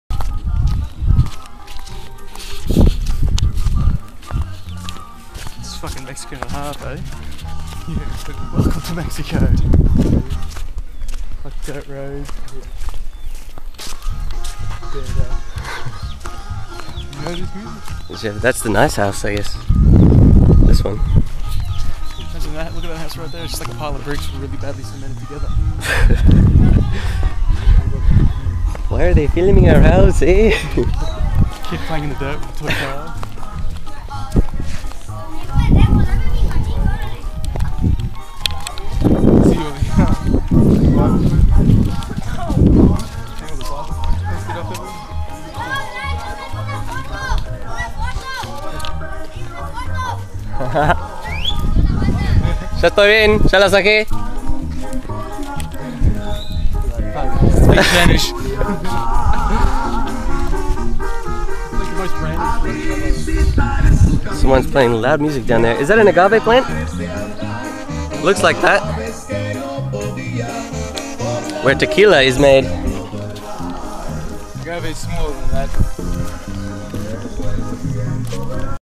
Walking in Manzanillo
Just taking a stroll from Buffalo Don to the shop for a bite  to eat.